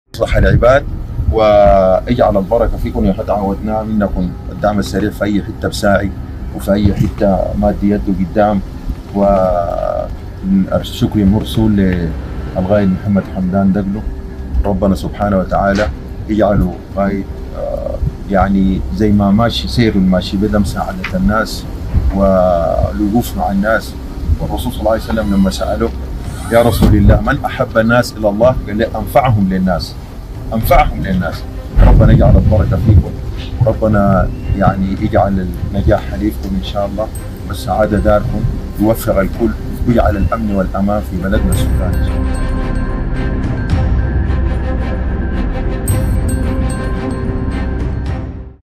تحفظ الجيش علي شيخ الأمين قسم الشارع الى نصفين
Stay updated with the news that matters to you
=== آخر الأخبار المرئية والمقروءة والمسموعة علي مدار الساعة ===